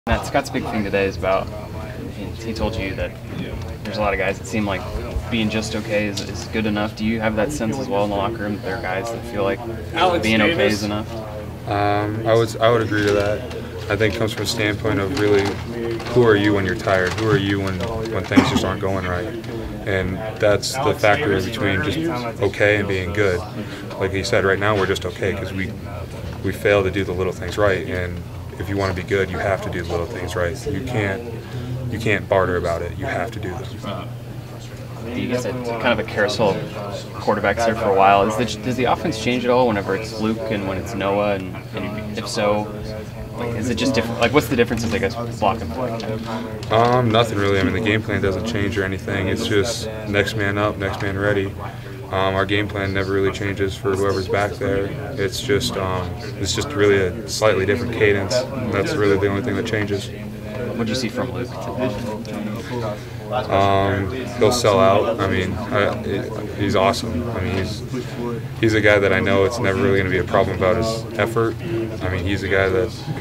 Matt Farniok Press Conference - Nebraska 31, Indiana 38 - - - Full Comments
Indiana was too much for the Huskers, as Nebraska fell 31 to 38 in Memorial Stadium.

The Huskers, who slipped to 4-4 overall and 2-3 in the Big Ten, piled up 514 total yards, including 294 passing and 220 rushing.

Indiana became bowl eligible by improving to 6-2 overall and 3-2 in the Big Ten.

Nebraska returns to Big Ten road action next week to take on Purdue. Kick-off between the Big Red and the Boilermakers is set for 11 a.m.